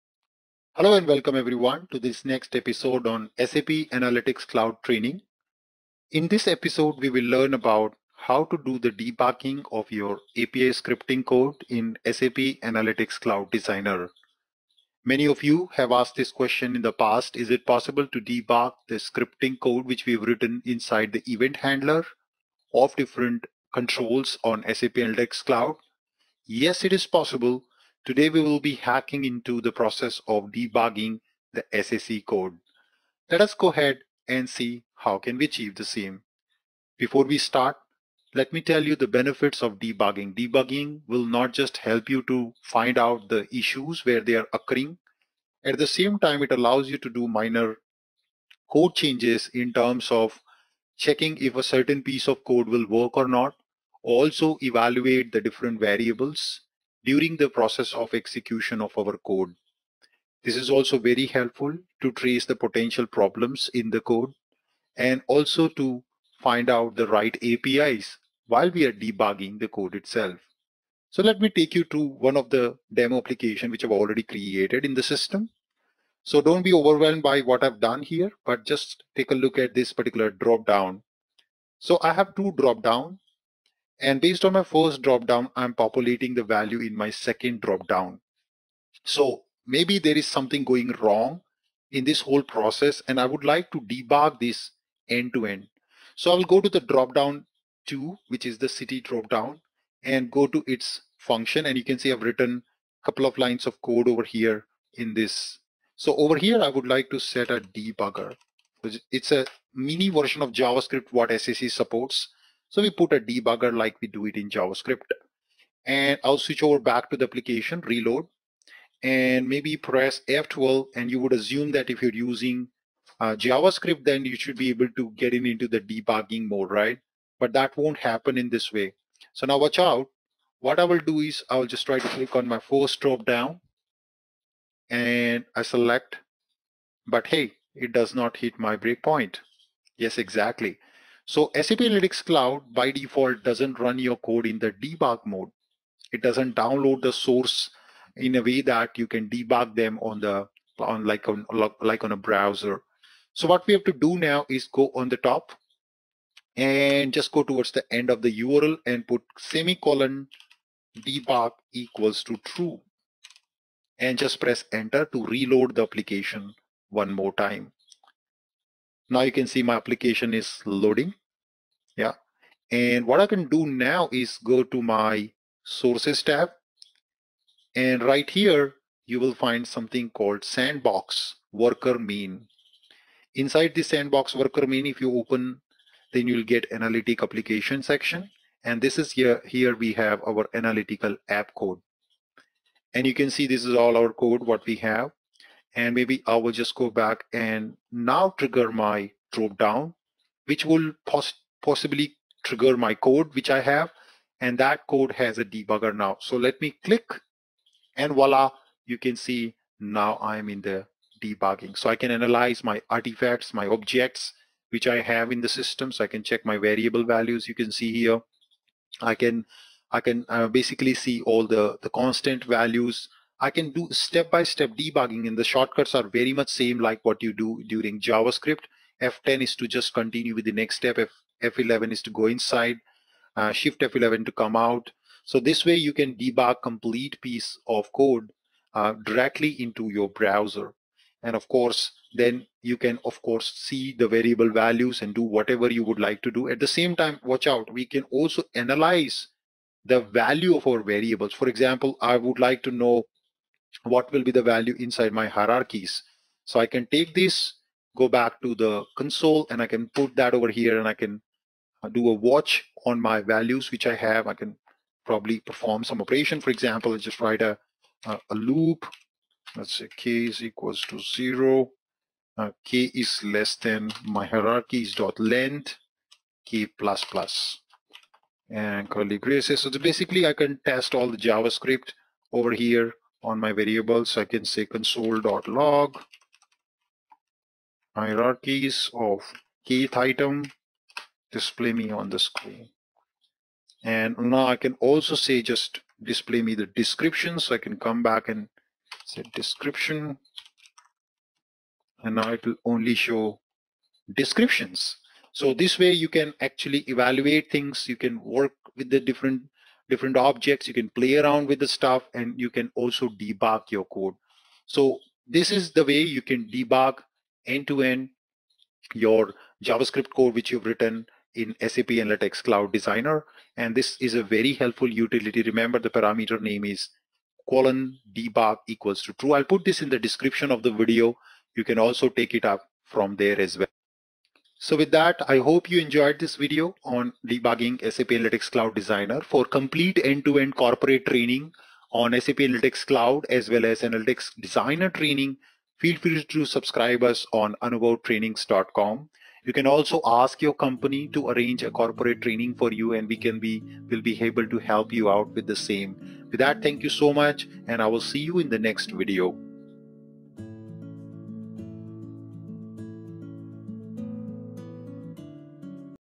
Debugging SAC Analytic Designer | How to debug Scripts in Analytics Designer Debug Mode
SAP analytics cloud Analytics Designer can be debugged easily by manually activating JS debugging option in browser url using ;debug=true option.
This was released with the latest Quarterly Release of SAP analytics cloud, allowing scripts to be edited in the browsers debugger also further allows quick test and Watch of variable values.
In this video we are going to discuss How to debug Scripts in Analytics Designer Debug Mode.

Debugging SAC Analytic Designer ,
How to debug Scripts in Analytics Designer Debug Mode,